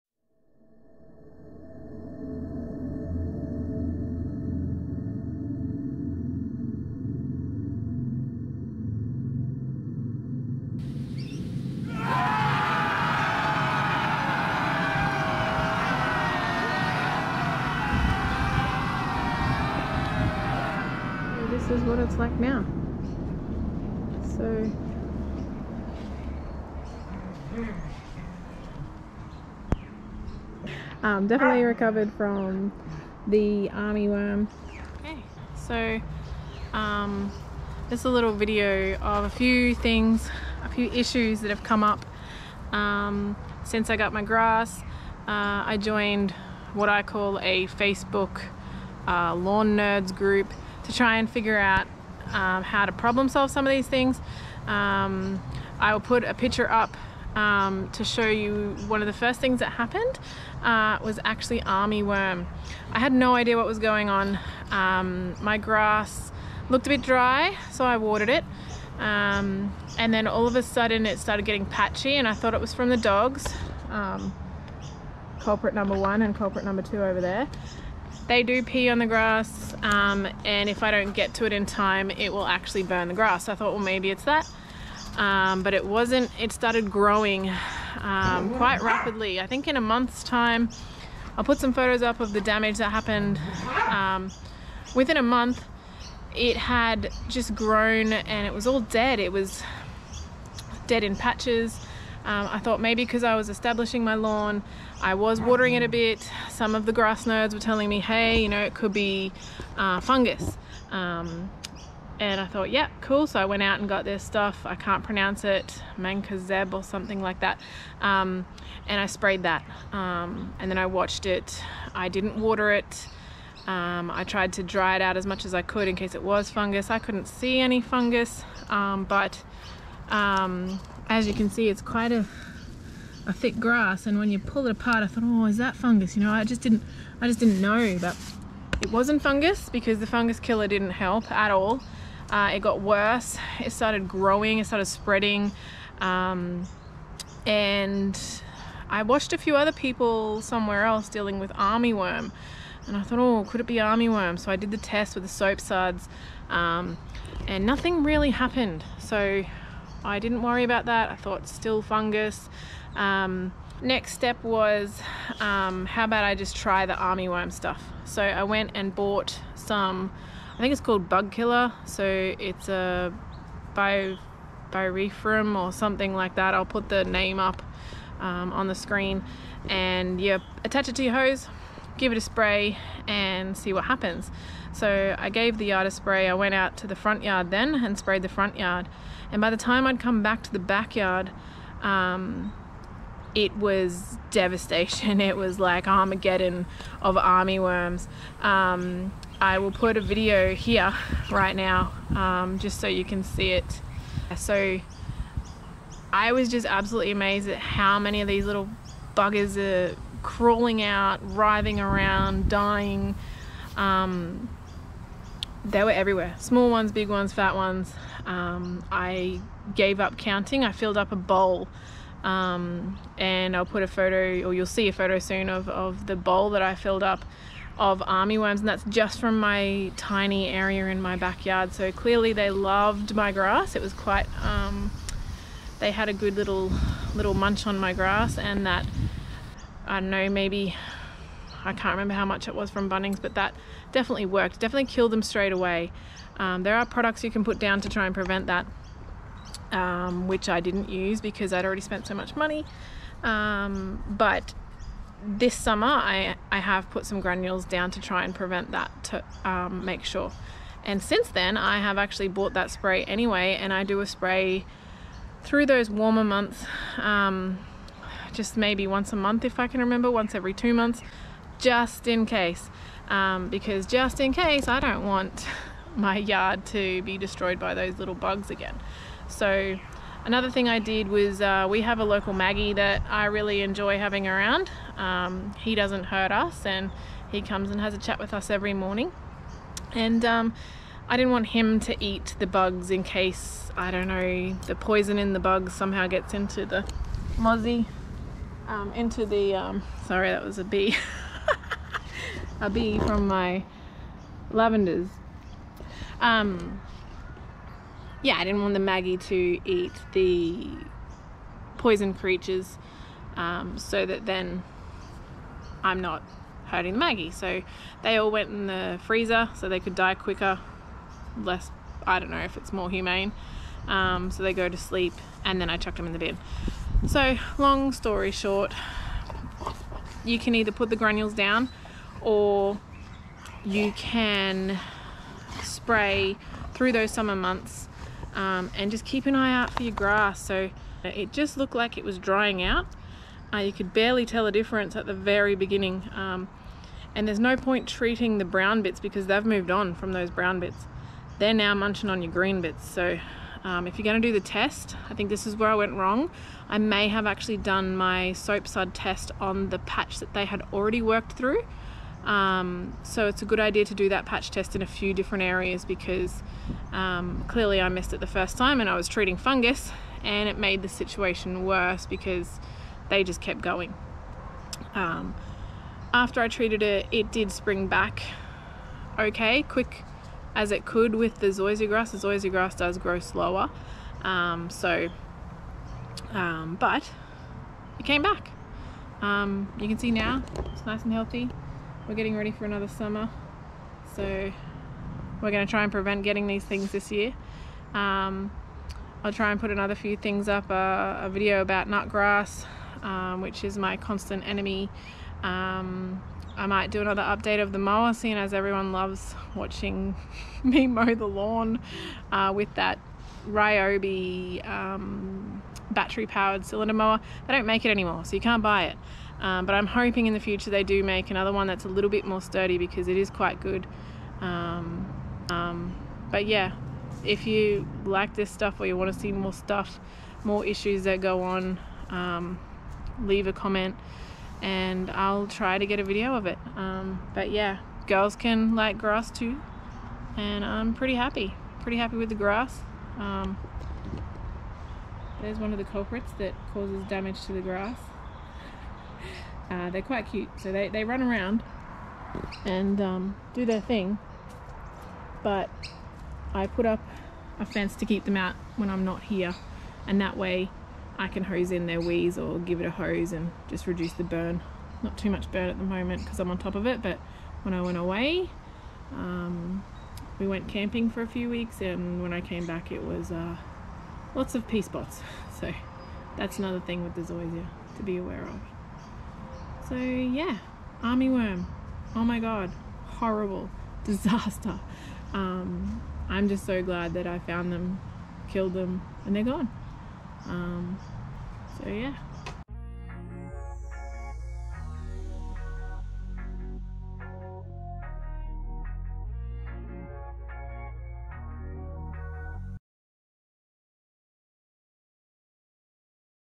How to Kill ARMY WORM! Lawn Grub Invasion! (on Zeon Zoysia) Sir Grange grass
The Battle is real! How to kill army worm, Lawn Grub Invasion on Sir Grange (Zeon Zoysia) grass.
Every summer, in the warmer months, the grass dies off in patches. Initially I thought it was a fungus so I tries Mancozeb But no It was the dredded Army Worm! (Lawn Grub). I even tried the soap suds test on the grass. but it didnt work. My beautiful Sir Grange Zoysia grass started to brown & die more and more, until I killed these little buggers.